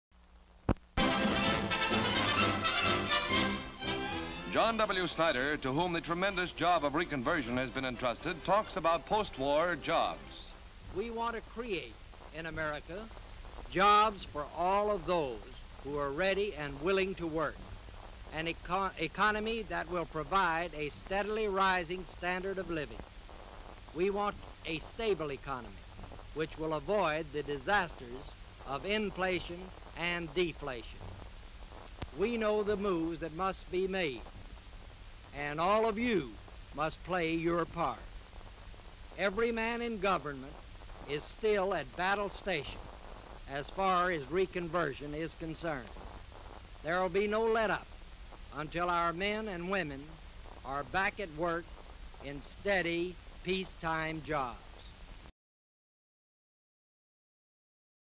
HD Stock Footage Government Promises Jobs For All Returning Servicemen
Newsreels 1945, August 16
John W. Snyder, Re-conversion Chief, states that the Government is committed to a program that will provide jobs for all who want to work..